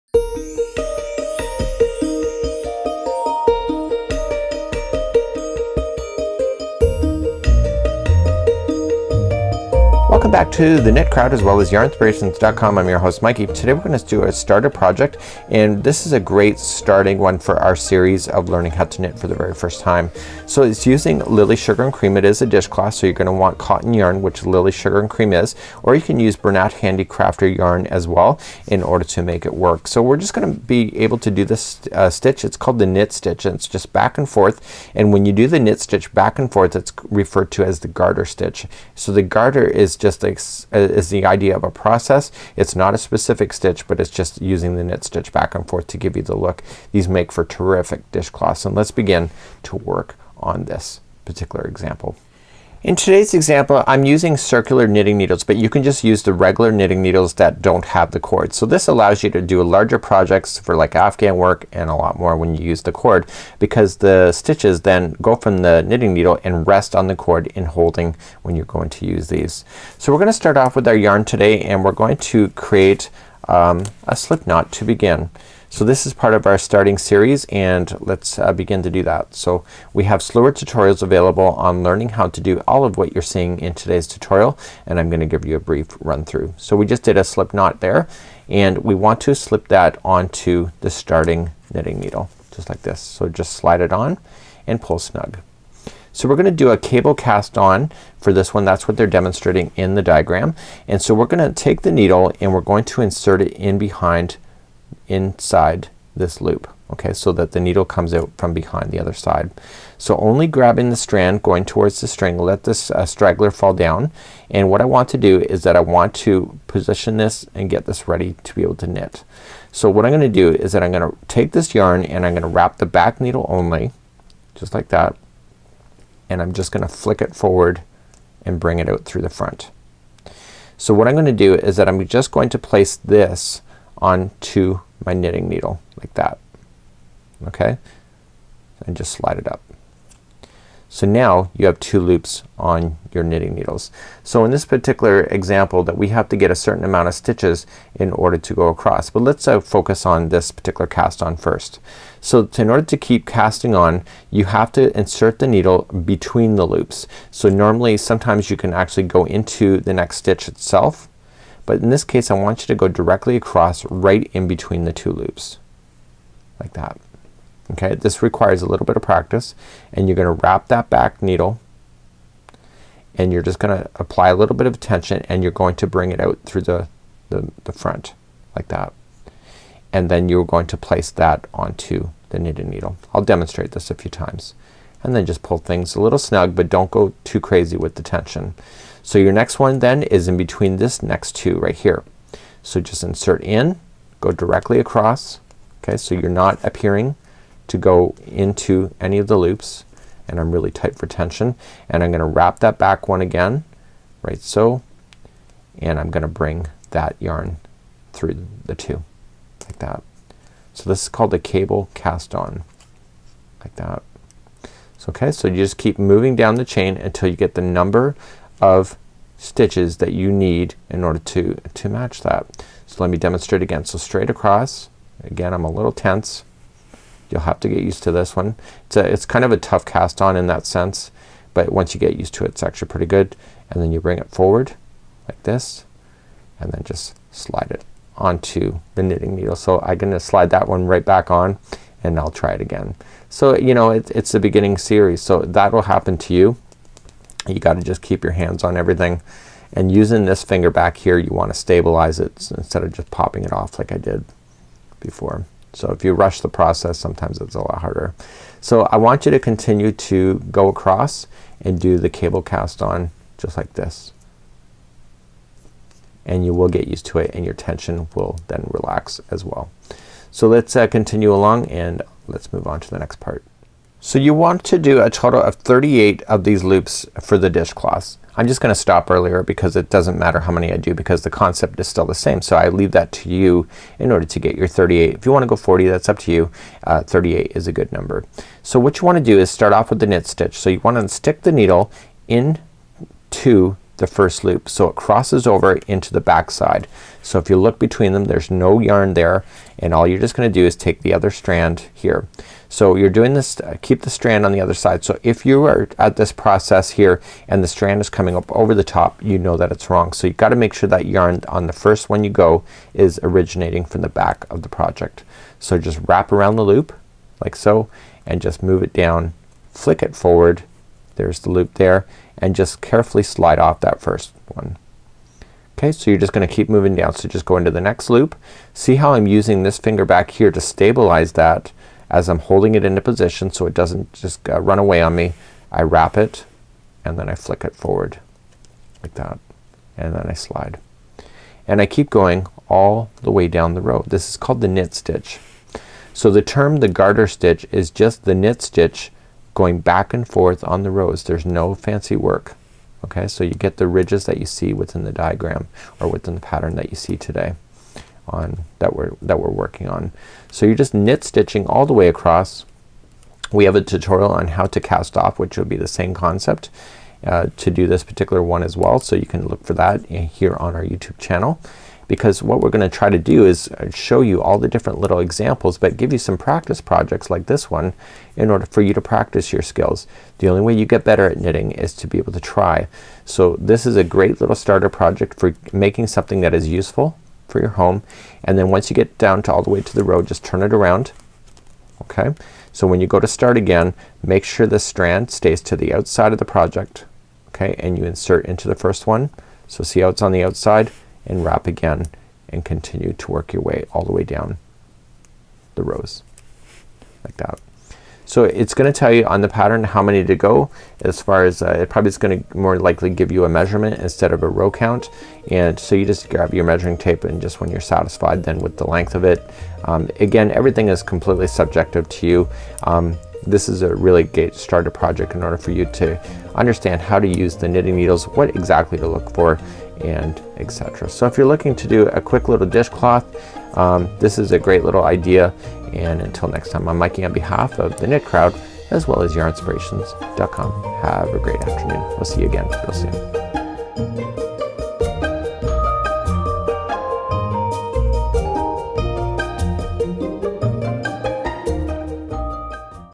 How to Knit a Dishcloth
Learn the basics of knitting with this simple pattern called the Basic Dishcloth. It is simple and using only the Garter Stitch as the technique. In other words, it's just the knit stitch back and forth using cotton yarns.